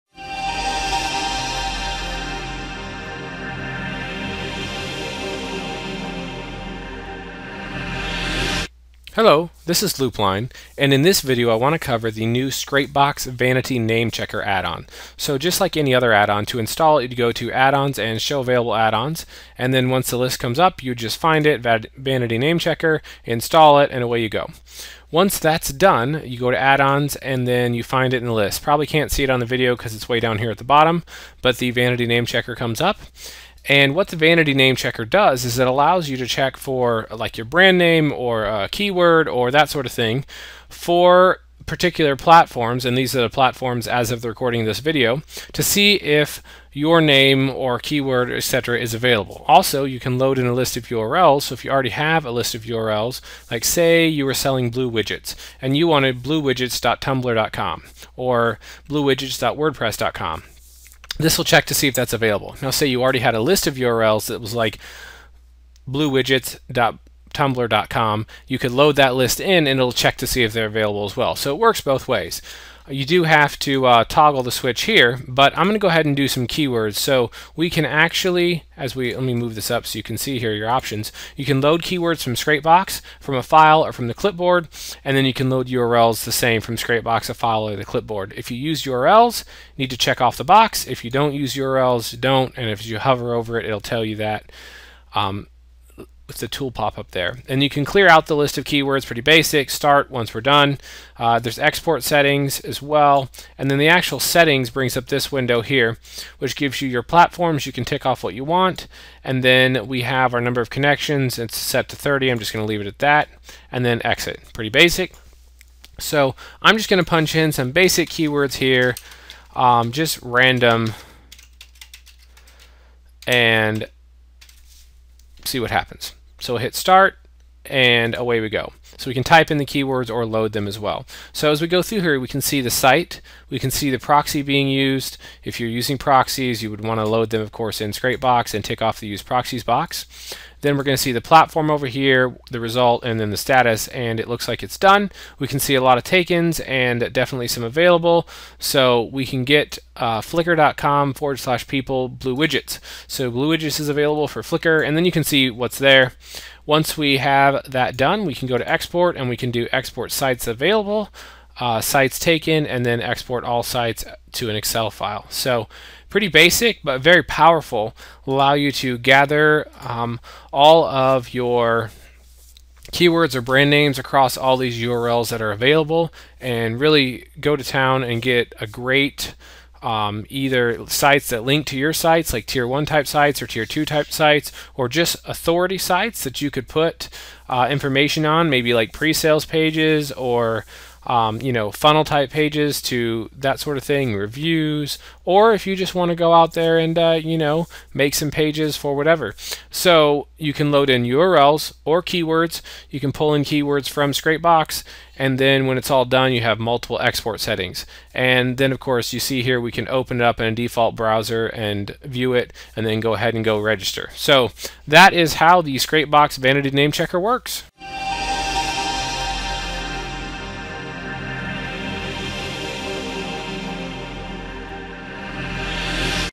Expired Web 2.0 for 2020 - Check if your keywords are available - Scrapebox Vanity Name Checker
The Scrapebox Vanity Name checker allows you to load in a list of keywords and it will check popular web 2.0 styles sites as well as others platforms.

This allows you to register your brand name or keyword for that platform and get it in the name/url. You can also load in a list of urls from these platforms and check to see if they are available.
The Vanity Name Checker is a free addon for scrapebox users.
Updated 1-3-2020